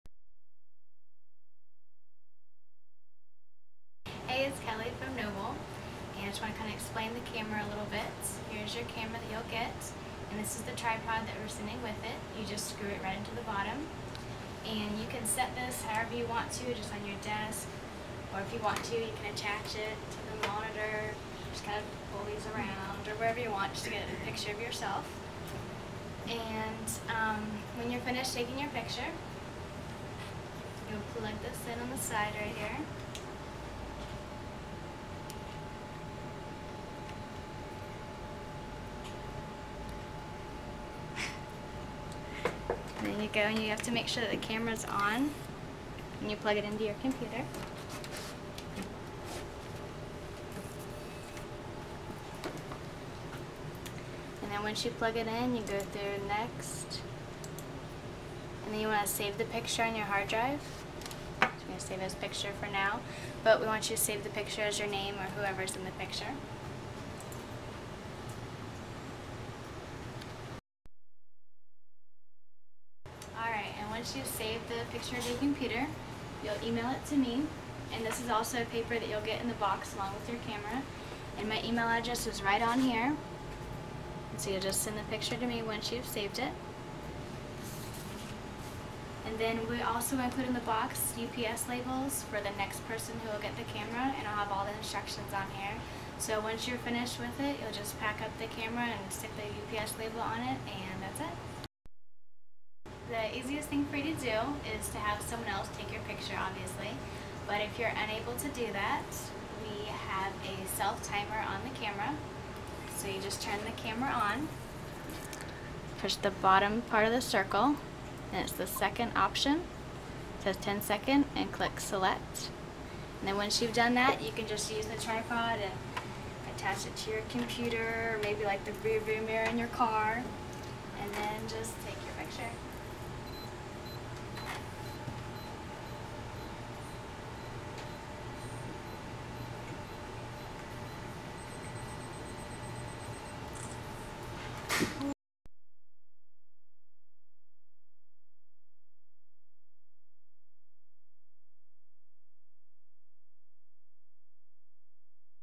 Inspire Paparazzi! Camera Instructions.MOV
A simple how-to on using the Canon Powershot SD1200 to take pictures for the Inspire publication.

Remember these important steps:
1.  Take candid photos of yourself and your day-to-day working environment including clients and office visits.   Have someone else take photos of you or use the auto-timer to take photos of yourself.
2.  Using the computer cord included in the UPS box, review the pictures you take.  Delete pictures you dont like; saving the pictures you like on your computer.
3.  Rename the photos to include your last name (For example: Anderson.jpg.) If you dont know how to do this, please call Theresa or Kelly at X 134.
5.  Send the camera to the next person on the list using the enclosed UPS label.